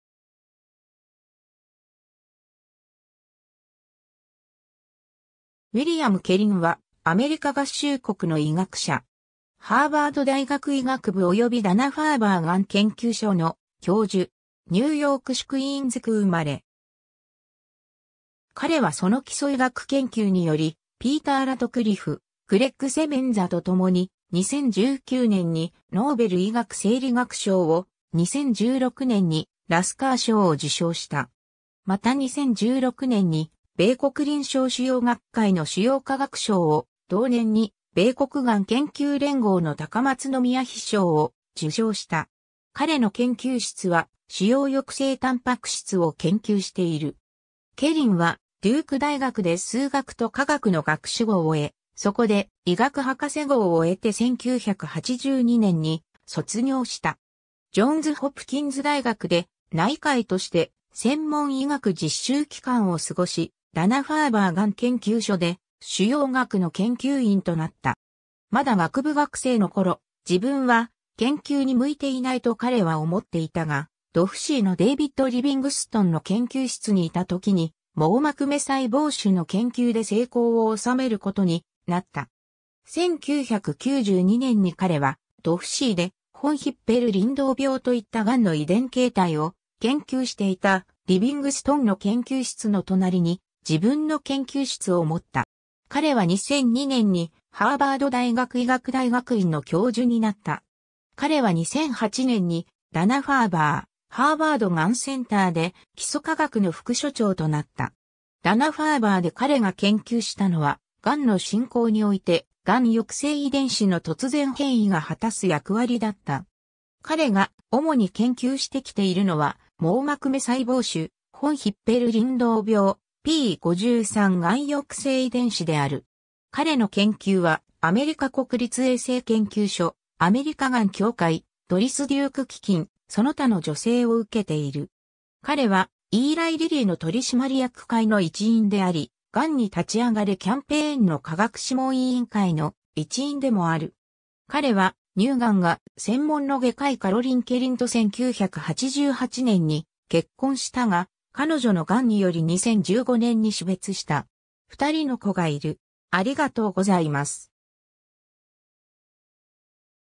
ウィリアム・ケリン
アルバート・ラスカー基礎医学研究賞受賞者
ハワード・ヒューズ医学研究所の人物
ウィリアム・ケリン（英: William "Bill" G. Kaelin Jr.、1957年11月23日 - ）は、アメリカ合衆国の医学者。
ハーバード大学医学部およびダナ・ファーバー癌研究所の教授。
ニューヨーク市クイーンズ区生まれ。
 彼はその基礎医学研究により、ピーター・ラトクリフ、グレッグ・セメンザとともに、2019年にノーベル医学生理学賞を、2016年にラスカー賞を受賞した。
また2016年に米国臨床腫瘍学会の腫瘍科学賞 (ASCO Science of Oncology Award) を、同年に米国癌研究連合の高松宮妃賞 (AACR Princess Takamatsu Award) を受賞した。
彼の研究室は腫瘍抑制蛋白質を研究している。
 ケリンはデューク大学で数学と化学の学士号を得、そこで医学博士号を得て1982年に卒業した。
ジョンズ・ホプキンズ大学で内科医として専門医学実習期間を過ごし、ダナ・ファーバー癌研究所 (DFCI) で腫瘍学の研究員となった。
まだ学部学生の頃、自分は研究に向いていないと彼は思っていたが、DFCI のデイヴィッド・リヴィングストンの研究室にいた時に、網膜芽細胞腫の研究で成功を収めることになった。
1992年に彼は DFCI で、フォン・ヒッペル・リンドウ病といった癌の遺伝形態を研究していたリヴィングストンの研究室の隣に自分の研究室を持った。
彼は2002年にハーバード大学医学大学院の教授になった。
 彼は2008年にダナ・ファーバー/ハーバード癌センターで基礎科学の副所長 (Assistant Director) となった。
ダナ・ファーバーで彼が研究したのは、癌の進行において癌抑制遺伝子の突然変異が果たす役割だった。
彼が主に研究してきているのは、網膜芽細胞腫、フォン・ヒッペル・リンドウ病、p53癌抑制遺伝子である。
 彼の研究はアメリカ国立衛生研究所、アメリカがん協会、ドリス・デューク基金、その他の助成を受けている。
 彼はイーライリリーの取締役会の一員であり、“がんに立ち上がれ”(Stand Up to Cancer) キャンペーンの科学諮問委員会の一員でもある。
 彼は乳癌が専門の外科医カロリン・ケリンと1988年に結婚したが、彼女の癌により2015年に死別した。
二人の子がいる。